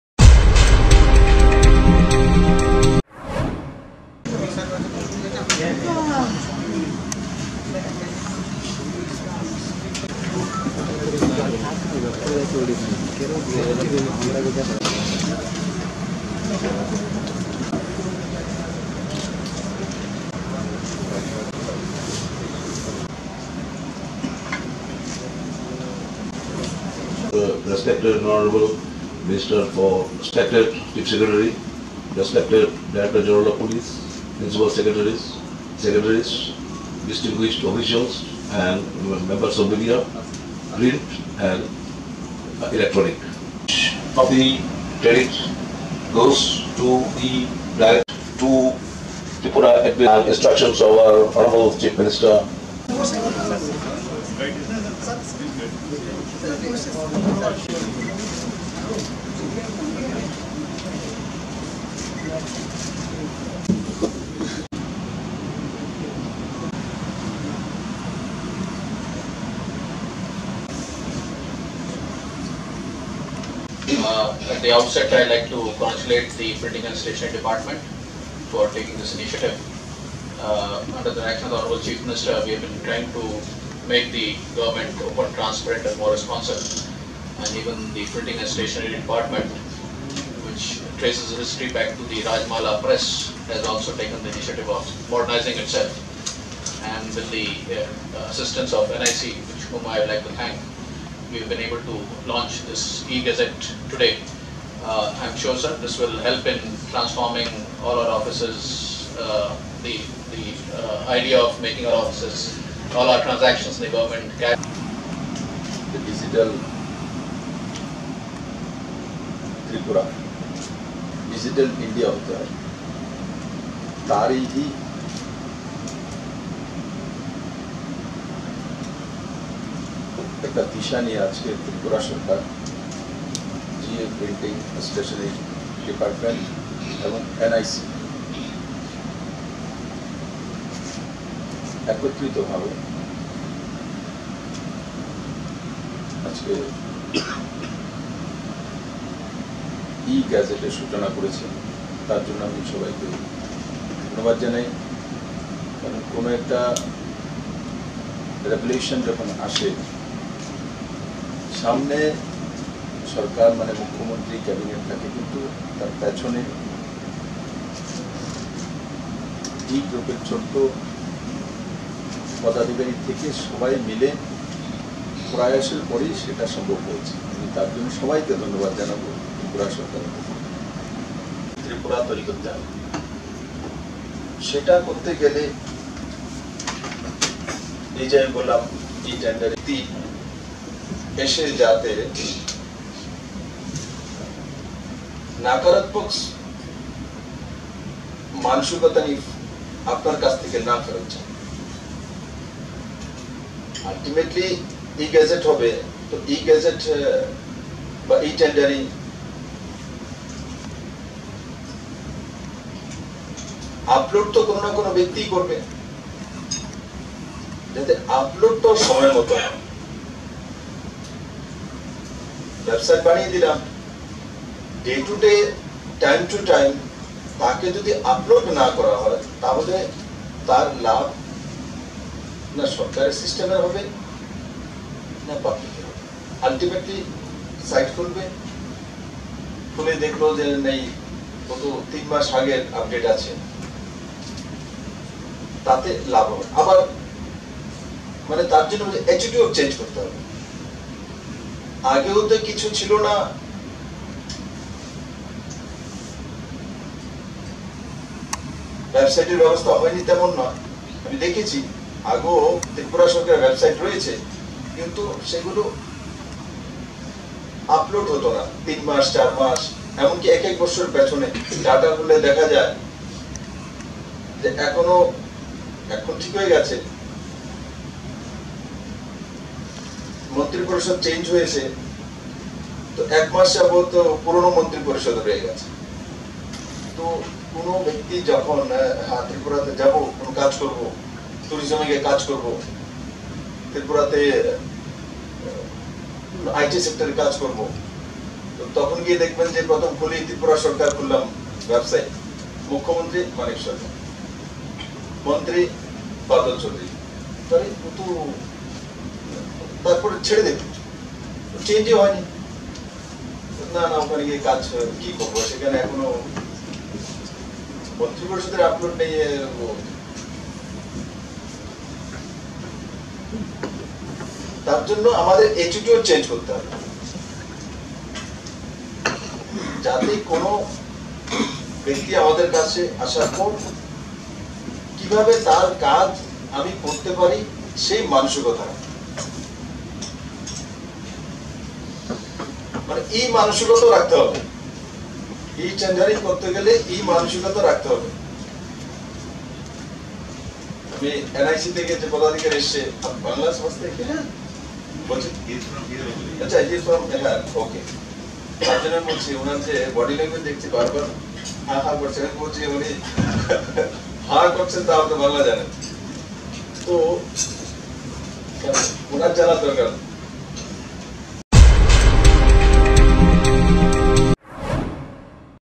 Launching of E-Gazette Tripura.
Launching of E-Gazette Tripura. Administrations (Printing&Stationery)Department Government of Tripura . Venue- Conference Hall No.2,Secretariat Building.